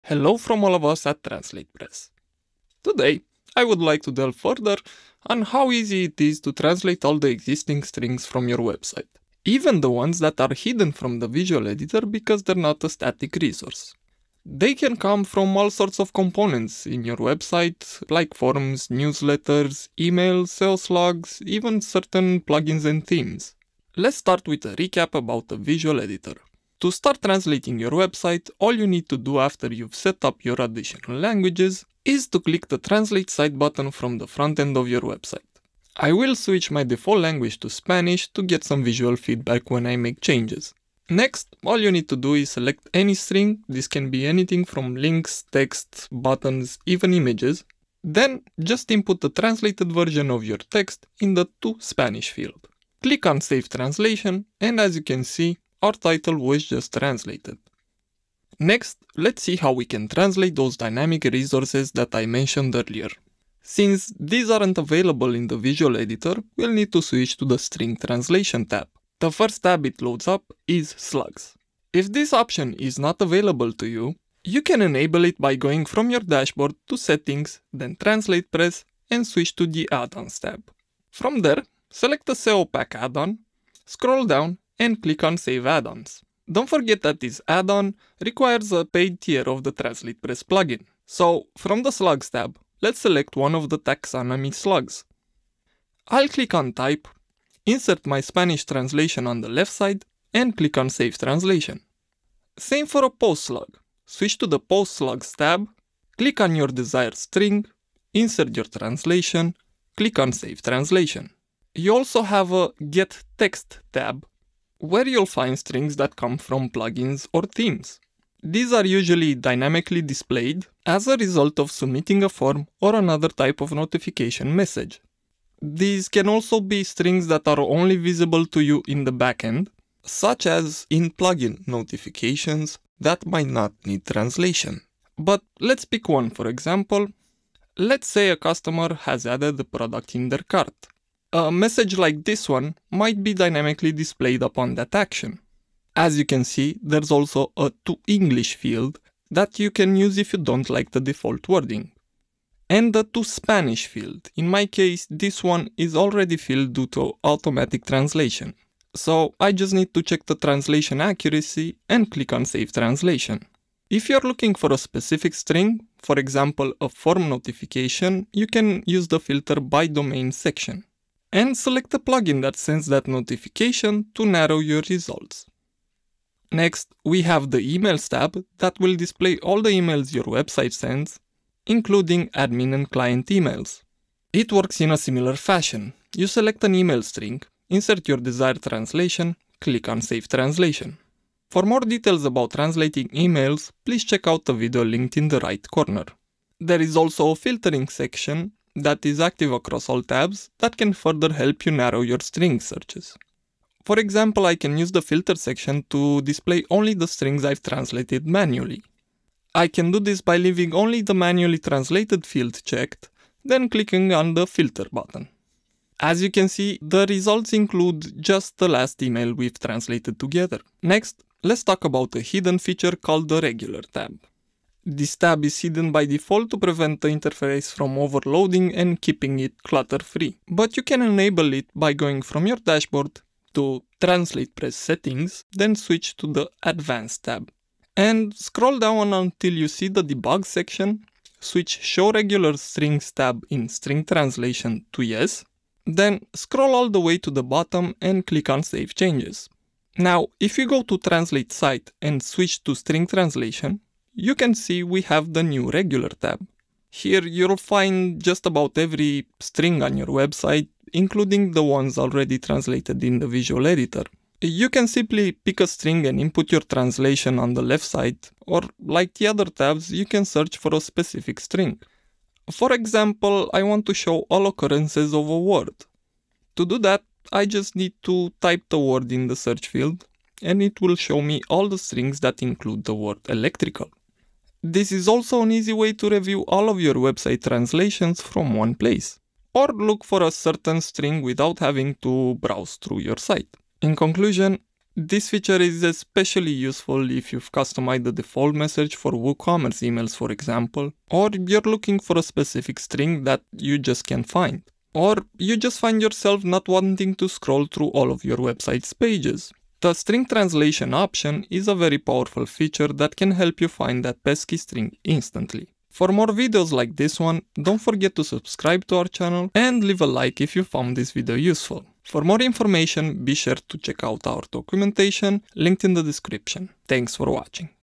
WordPress String Translation (How to Translate Any Strings on a WordPress Site)
Learn how to translate any WordPress string on your site, from basic text and images to hidden content and even emails using TranslatePress.

TranslatePress is a freemium WordPress plugin that turns your website multilingual by letting you translate all of your website content both through its visual editor (for front end content) and the string interface (for all other types of content).

Chapters:
00:00 Intro
00:24 How to translate basic strings?
00:58 How to translate slugs?
01:52 How to translate hidden strings?
02:56 How to translate emails?
03:41 How to review all translations?
04:54 Conclusion

Join our Facebook TranslatePress Community and get support from others just like you